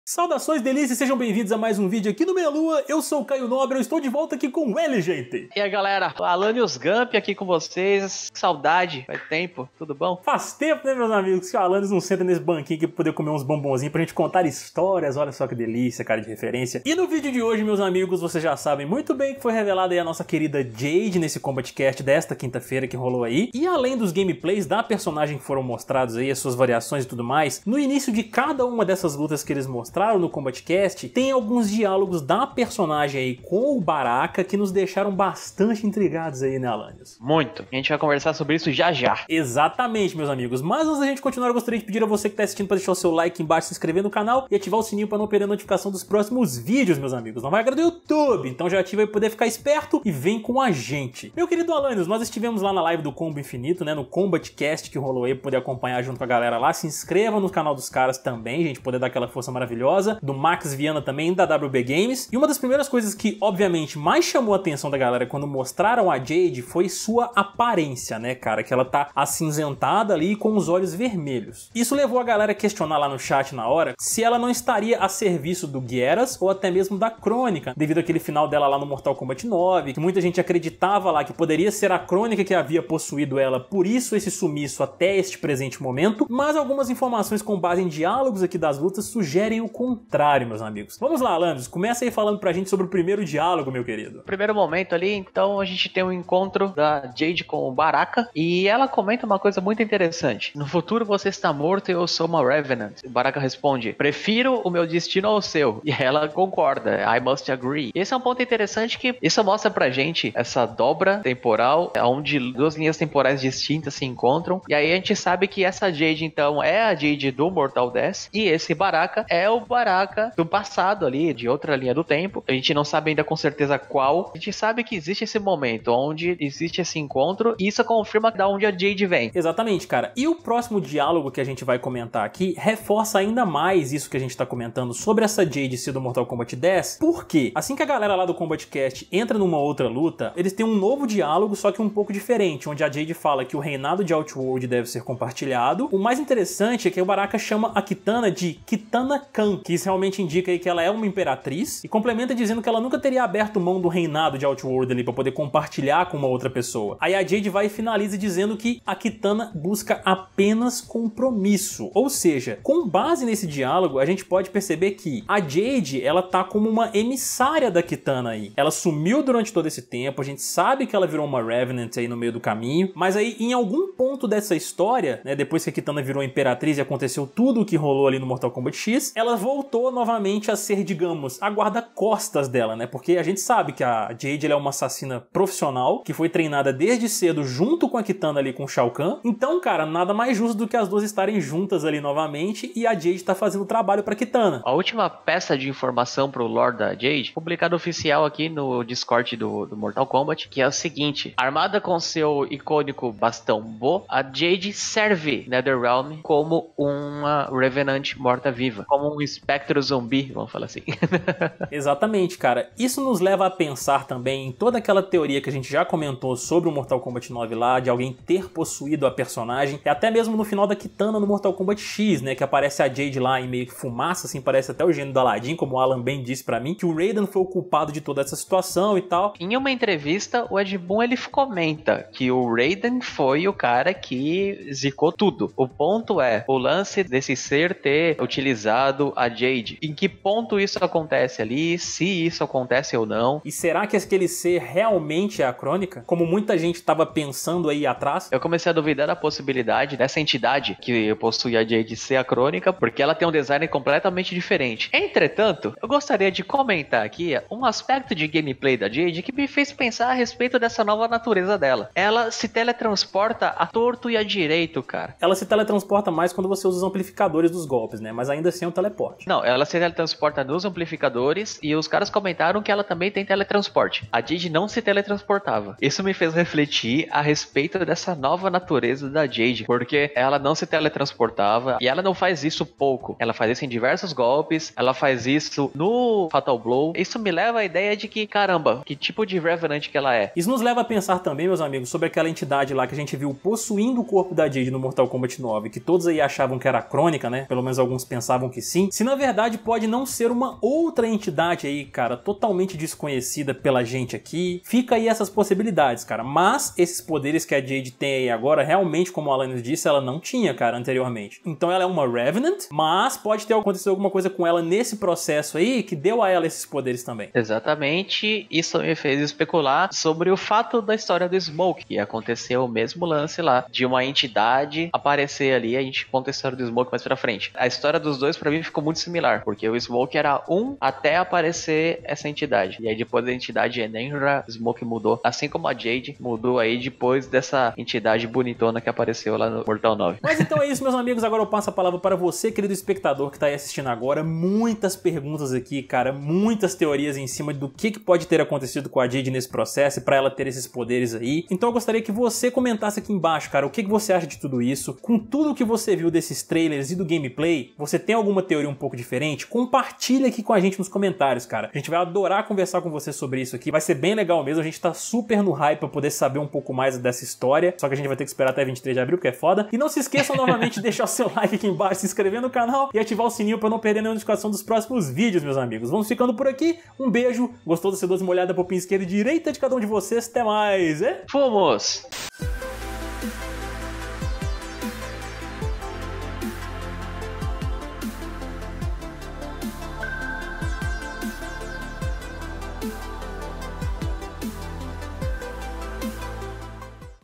MORTAL KOMBAT 11: JADE SERVE A KITANA? DE ONDE VEM SEUS NOVOS PODERES?
No vídeo de hoje comentamos sobre a aparência e especulamos sobre a participação da nossa querida Jade em Mortal Kombat 11. Será ela uma serva da Kitana, da Kronika?